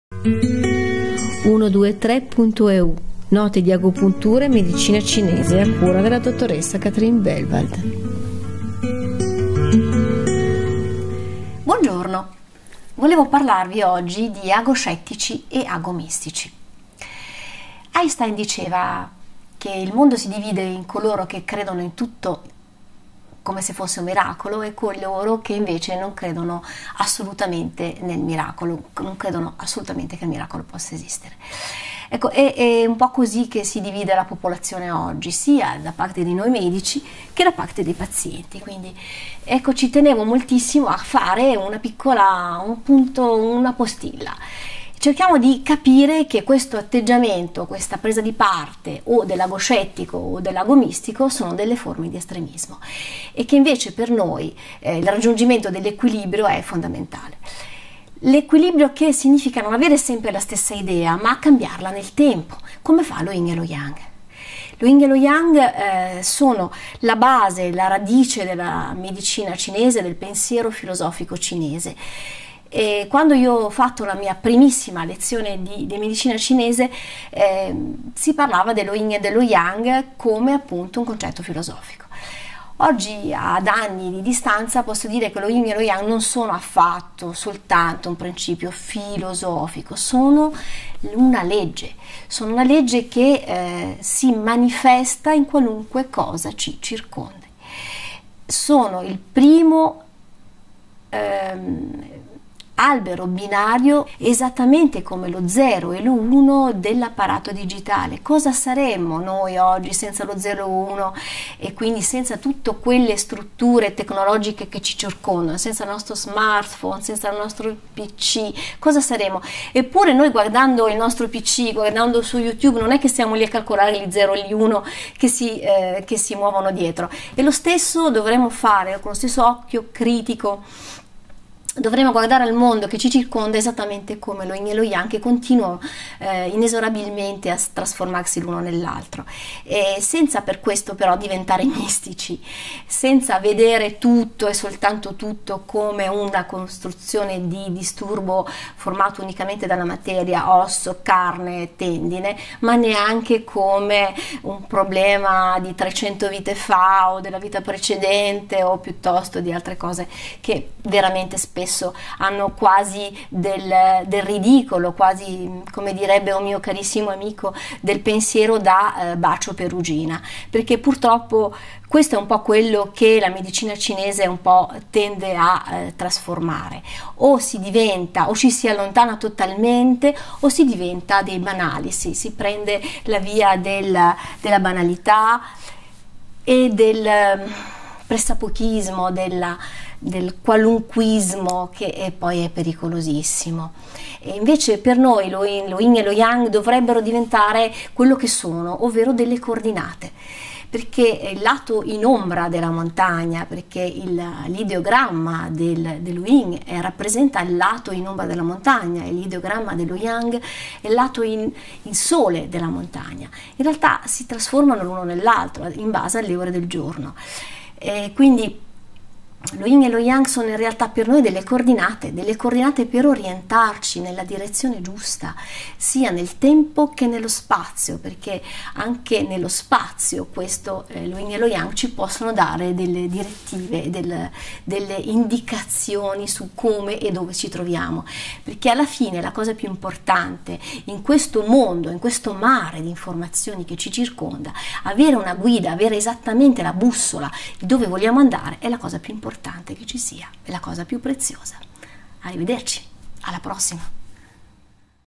Agoscettici ed agomistici: il concetto di Yin e Yang
Albert Einstein diceva “Il mondo si divide in quelli che vedono in tutto un miracolo e quelli che non vedono in niente un miracolo” lo stesso potremo dire in merito all’agopuntura. Infatti sia per i pazienti che per i medici l’agopuntura riceve lo stesso identico approccio e fama del sopra menzionato miracolo.